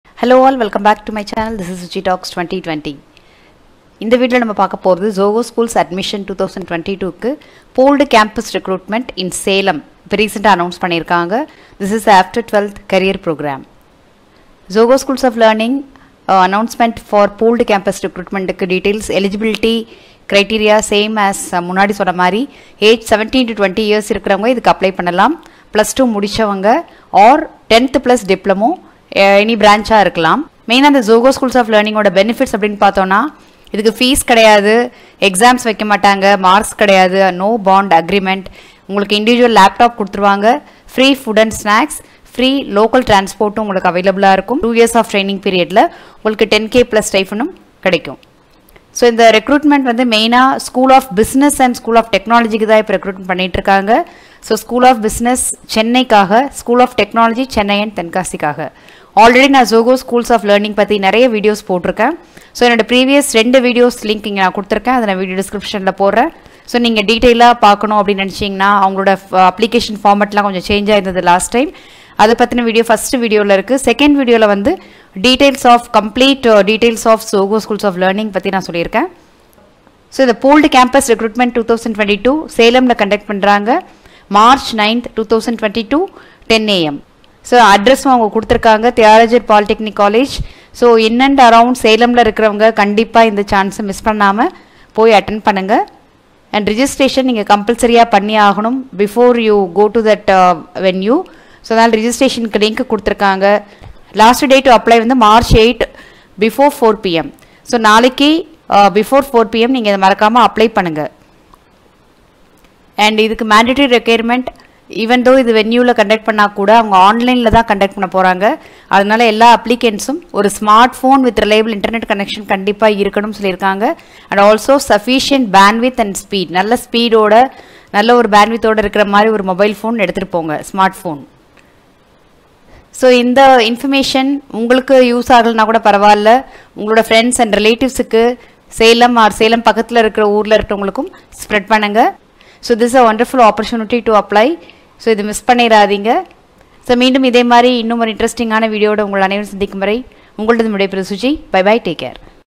ZOHO schools Admission 2022 for XII/X + Diploma Students| No Fees | Stipend | In Salem
Welcome to SuchiTalks 2020!!

Link to apply for ZOHOSChools ZETA Exam
Registration is Mandatory.

March 8th, 2022 before 4 pm

Also watch the following videos to know about the complete process

Important Dates:

Last Date to Apply: Mar 8th 2022 4pm

ZETA (Entrance Test) Date : Mar 9th 10am

Venue :
Test Venue: Thiagarajar Polytechnic College, located in Sona College of Technology, Salem, Tamil Nadu, India.

** ZOHO Schools of Learning is the Academy run by ZOHO Corp.
** ZOHO school gives training to the students to equip them to work in Software Profession.
** This is not eligible for College students.
** Any one from All over India can apply for this program.

Selection will be based on your skill based achievements like
1. If you have coding or programming knowledge and done some programming in
Hackerrank/hackerearth/GitHub websites or any other websites

2. Any technical questions answered and approved in websites
like stackoverflow.

3. If you have any blog or participated in Essay writing competitions

4. If you have done creative Art/Painting/Logo/animation

Also watch the videos published in ZOHO schools YouTube channel.Link given below.

zohoschools 2022 Admission!!

Zoho Schools now operates only from two locations in Tamil Nadu, India. Applications are welcome from all over the country!

16 years
1000+ students

10% of Zoho's employee count

About Zoho Schools

** What they teach is relevant to real software careers

** Based on Skillsets and abilities more than for certificates(paper credentials)

** No Fee

** But you get Stipend starting Rs.10000 (throughout the course of study)

** AFter the successful completion of the course,
you are automatically inducted as an Employee of Zoho Corporation (Leader in Cloud based S/w Products)

There are three areas

1. Zoho School of Technology

  -- S/W Engineering and Product Development

2. Zoho Schools of Advanced Study

  ** Who are passionate to develop the knowledge in two fields not just depth in one
  ** This is similar to students doing major and minor in a standard higher education like degree

  ** The two fields can be from three (Tech, Business and design)

3. Zoho School of Design

   ** Students with a keen interest in art and an eye for Aesthetic detail, A finishing school for user experience professionals

4. Zoho Schools of Business

   ** Mix of Excelllent support,effective marketing, and efficient sales

Qualification:

Age should be between 17 to 20
Academic qualification should not be greater than
XII or X + Diploma or XII + Diploma

Eligibility Criteria :

1. You should have been born BETWEEN April 1, 2002  AND Dec 31, 2005.
2. Your highest educational qualification can be Class XII or Class X + Diploma/Class XII + Diploma only.
3. Students in their final year of schooling, or final semester of their Diploma can appear for the examless evaluation.
4. Students who have already completed more
 than a year of any degree program (B.Tech, B.E, B.Sc, B.Com, etc) at the time of applying, will NOT be allowed to sit for the examless evaluation.